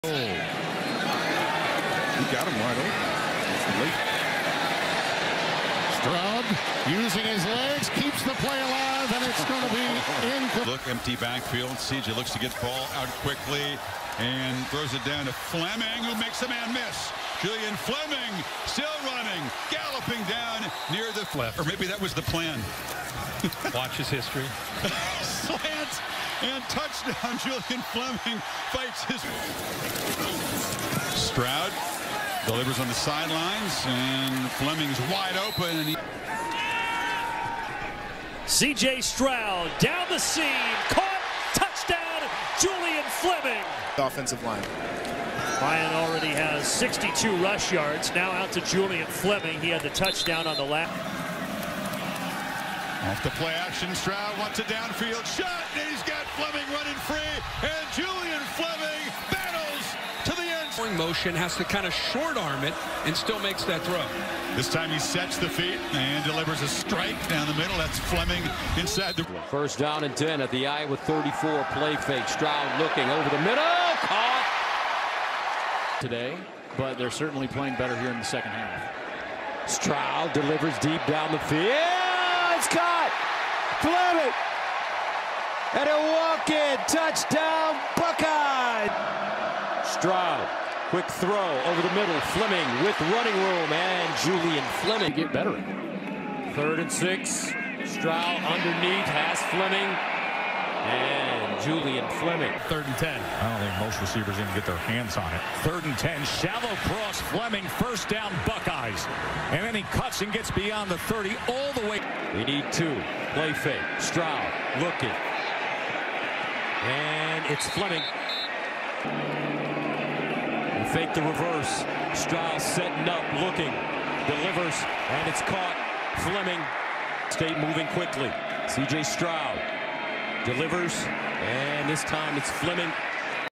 Julian Fleming || Ohio State Buckeyes Wide Receiver || 2022 Junior Highlights
Julian Fleming 2022 season highlights. 462 Receiving Yards, 29 Receptions, 6 Receiving Touchdowns.